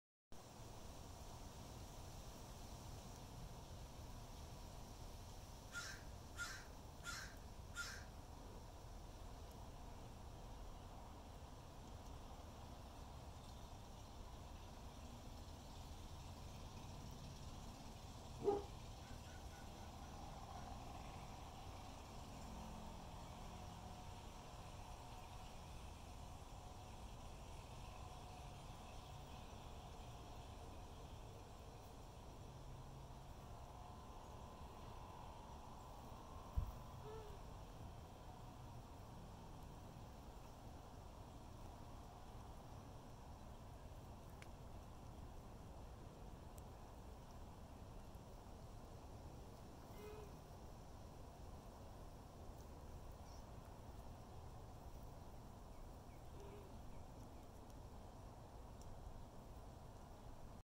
Kittens EP17
Mother cat and kittens are in the sleeping mode.